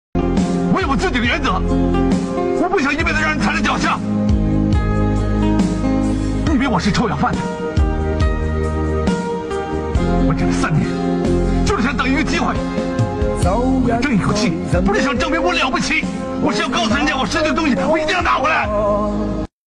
Chinese Bear Shouting meme template
English Translation:

I have my own principles.
I don’t want to be stepped on by others all my life.
Do you think I'm a beggar?
I waited three years.
I just want to wait for an opportunity.
I want to take a breath.
Not trying to prove that I'm awesome.
I want to tell people.
What I lost.
I must get it back.